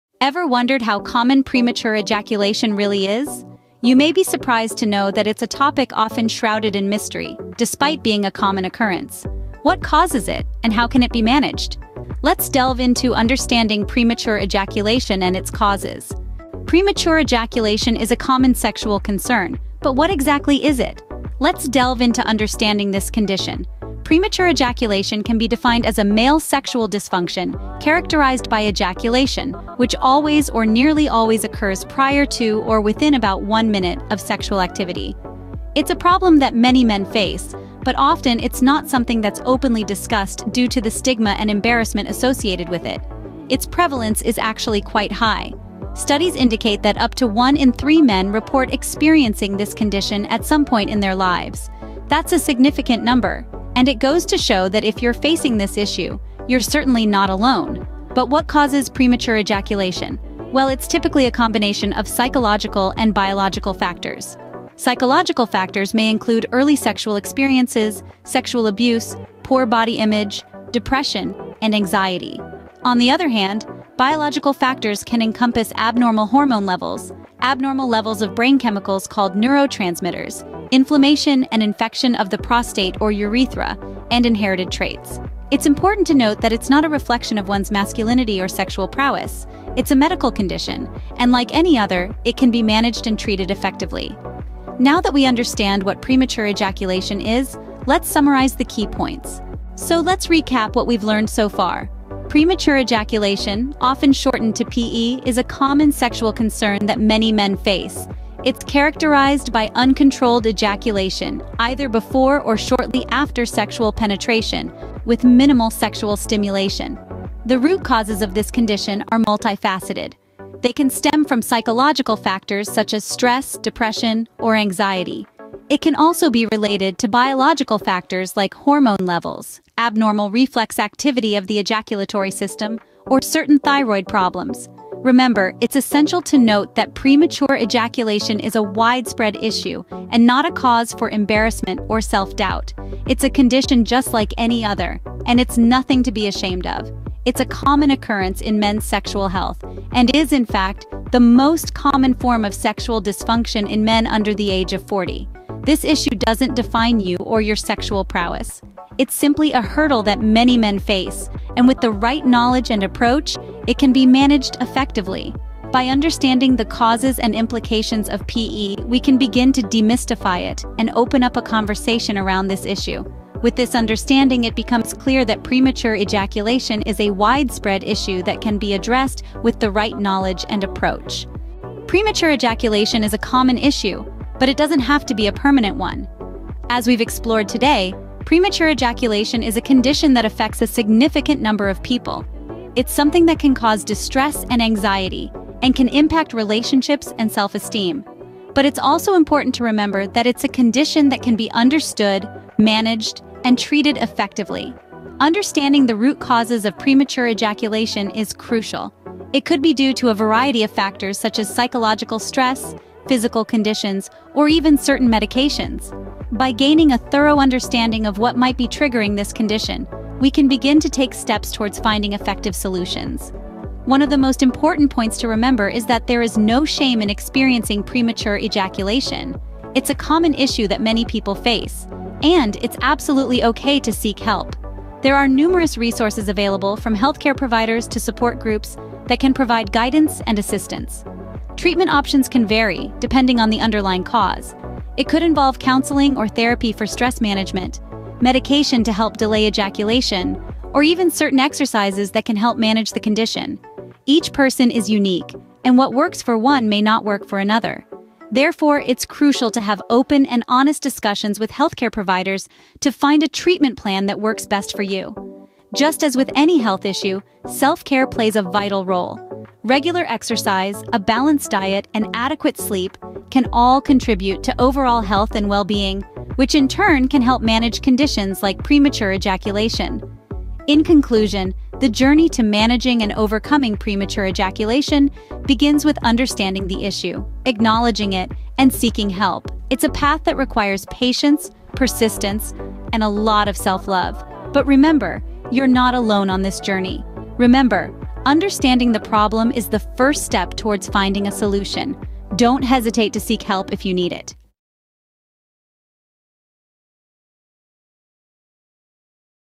Premature Ejaculation Medicines & Usage | Treatment
Mastering the Mystery: Premature Ejaculation Medicines & Usage by Roy's Workspace

OUTLINE:

00:00:00 Opening Question
00:00:16 Understanding Premature Ejaculation
00:01:39 Key Points Summary
00:03:01 Conclusion